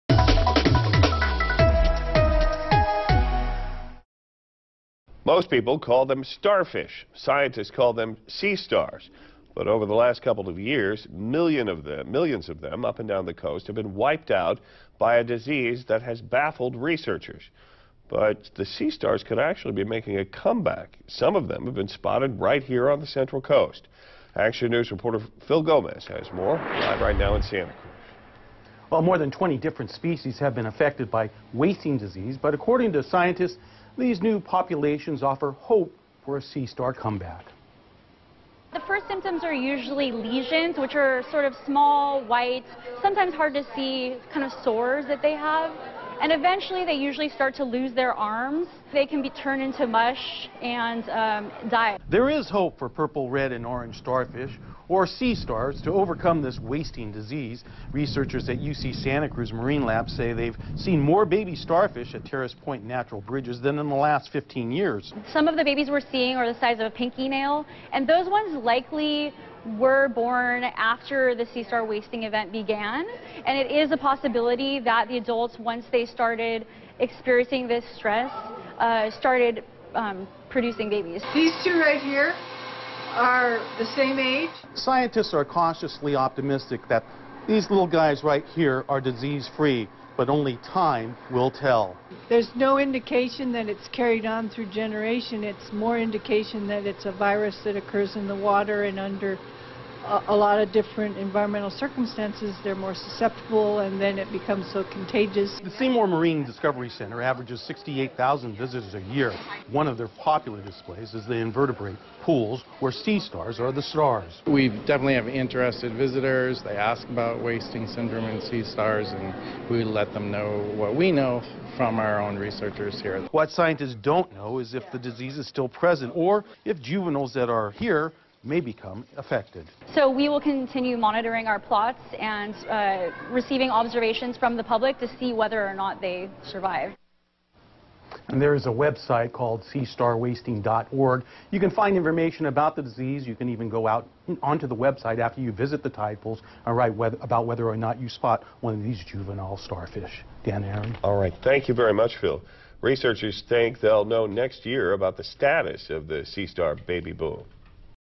Starfish making a comeback, babies spotted in Monterey Bay
There is a baby boom of sorts happening that has scientists hopeful of a come back for sea stars infected with a disease that's wiped out millions of starfish up and down the West Coast.